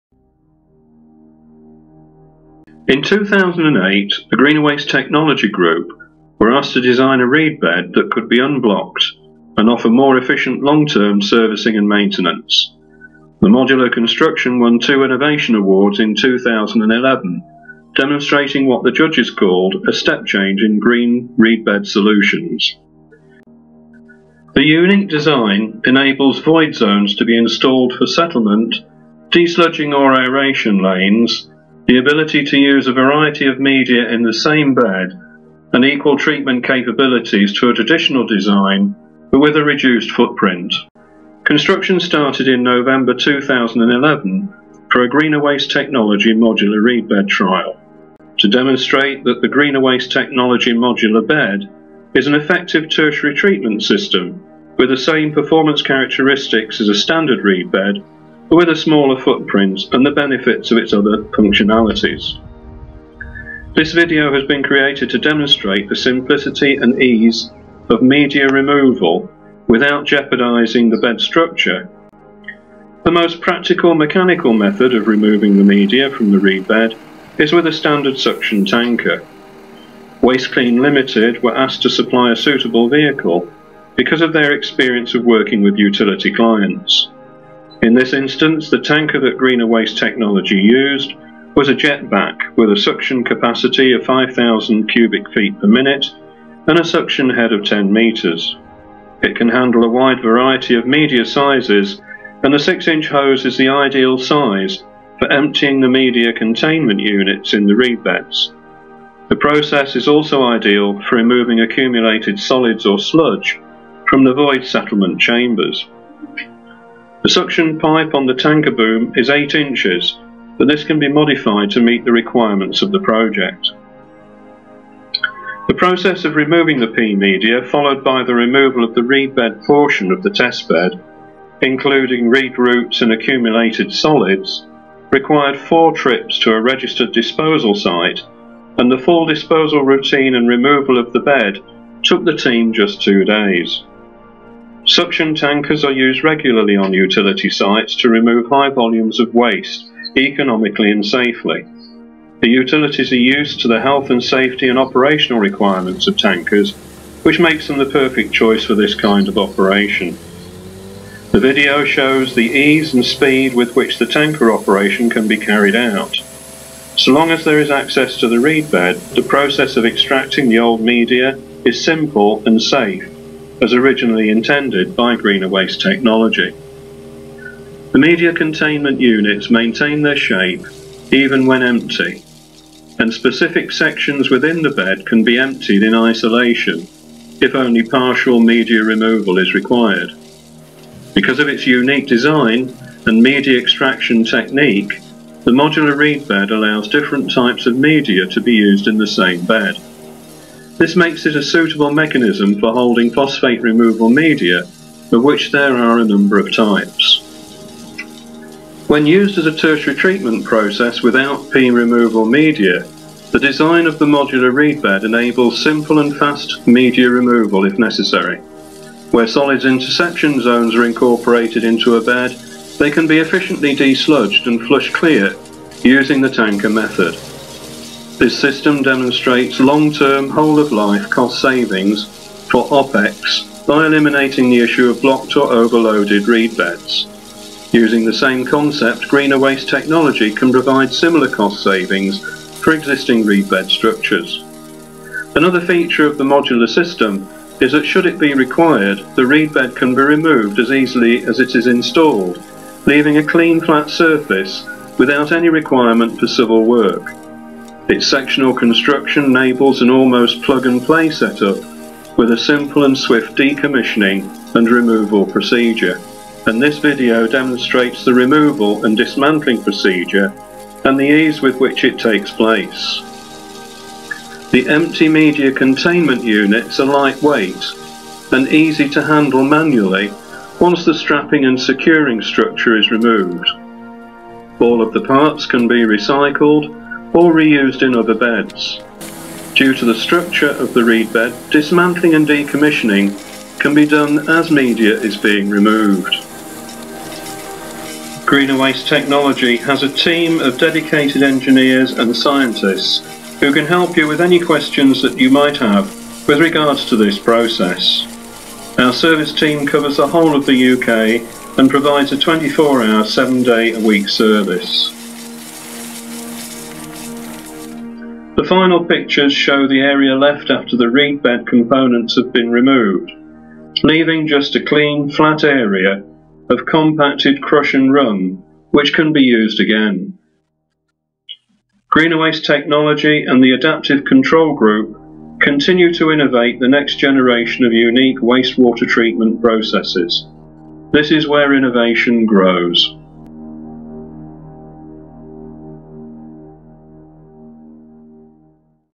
GWT modular reed beds
View the innovative modular reed beds from Greener Waste Technology and see how simple and easy tertiary treatment can be.